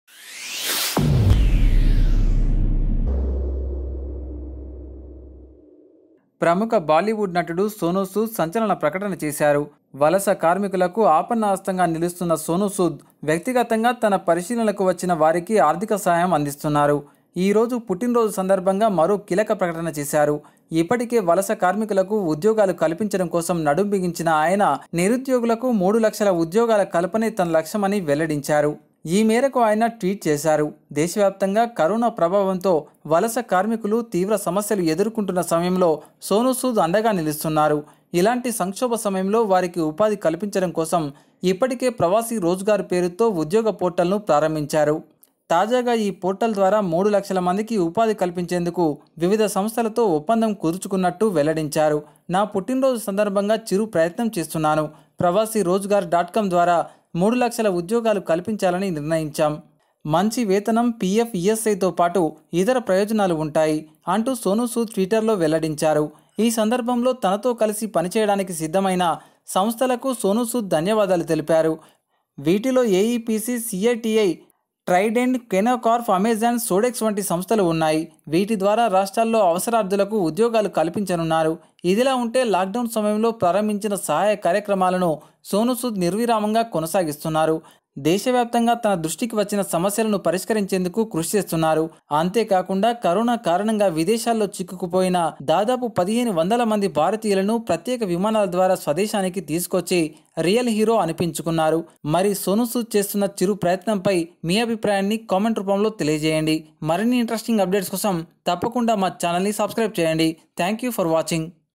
3 లక్షల ఉద్యోగాలు ఇప్పించేందుకు సిద్దంగా ఉన్నాం సోనూ సూద్ గ్రేట్ / Sonu Sood Jobs 3 Lakhs / ESRtv
3 లక్షల ఉద్యోగాలు ఇప్పించేందుకు సిద్దంగా ఉన్నాం సోనూ సూద్  గ్రేట్ / Sonu Sood Jobs 3 Lakhs / ESRtv